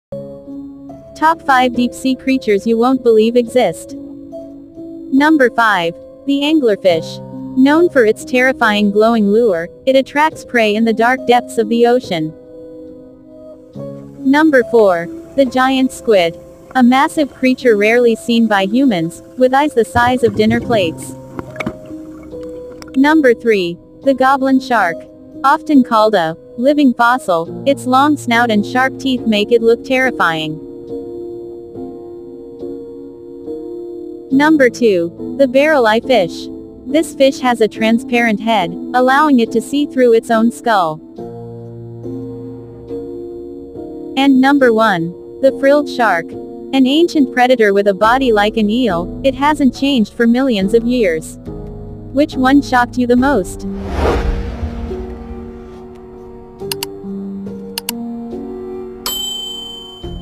Top 5 Deep Sea Creatures You Won’t Believe Exist !!!
Dive deep into the ocean and discover the Top 5 Deep Sea Creatures You Won’t Believe Exist! From glowing predators to transparent-headed fish, these creatures are both terrifying and fascinating.